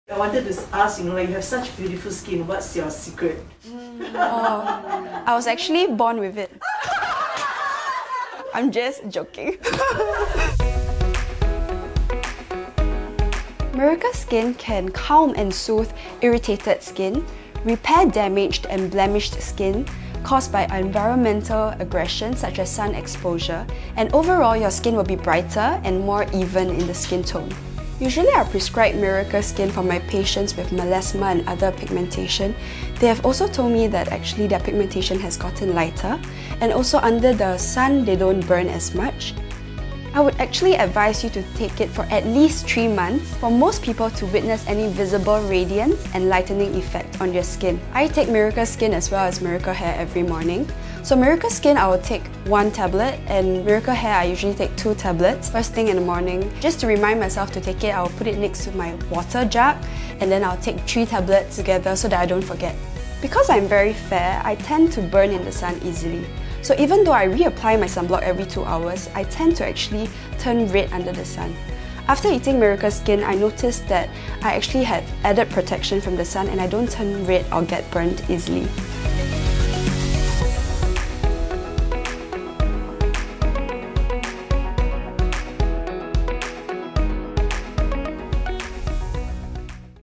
Dr Hoe Ying Min's personal experience with Miriqa® Professional Skin Nutrition Supplement.
Prolonged UV exposure is linked to increased risk of skin cancer, sunburns, premature skin aging (photo-aging), increased wrinkles, pigmentations. ⁠
However, regular application of sunblock can be a challenge with non-compliance, the inconvenience of reapplying every 4 to 6 hours and other factors such as perspiration reducing the effectiveness of the sunblock coverage. ⁠
Miriqa® Skin is a great complement to your sunblock in providing sun protection from the inside out and boosting your skin's resistance to the sun, while enjoying all your favorite activities like cycling, rollerblading and hiking. ⁠
Hear from the gorgeous and knowledgeable Dr Hoe Ying Min on her personal experience with Miriqa® Professional Skin Nutrition Supplement and the patients whom she prescribes the supplements to! ⁠
Invest in your skin. Filters are great, but great skin is better.⁠